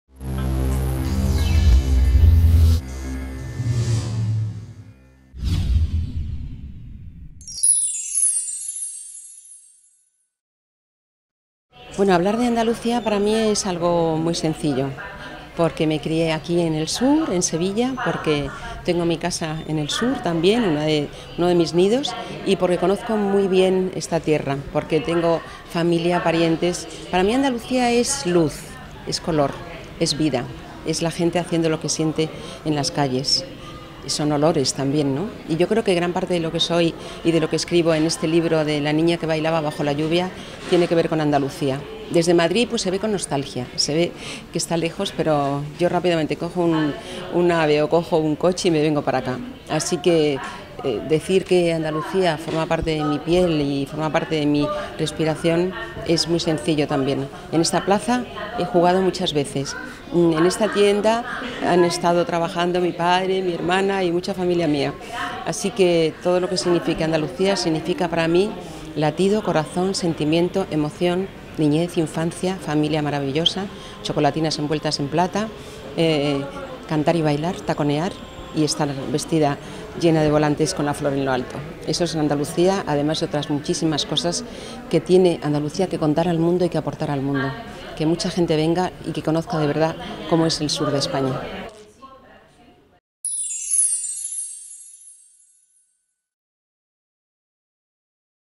HDL El granito de luz de la cantante Paloma San Basilio
Paloma San Basilio nació fuera de Andalucía pero pasó toda su infancia en el sur, en Sevilla concretamente, y aún se siente ligada a la tierra por ella misma y por sus familiares. Para la cantante, Andalucía es sinónimo de "luz, color, vida y gente haciendo lo que siente" y por ello, aunque viva fuera, en cuanto siente nostalgia no duda en acudir a Andalucía porque lo tiene claro: "Forma parte de mi piel y forma parte de mi respiración". Por último, recuerda que las "muchísimas cosas que tiene Andalucía que contar y aportar al mundo" e invita a que la gente venga y "conozca de verdad cómo es el sur de España".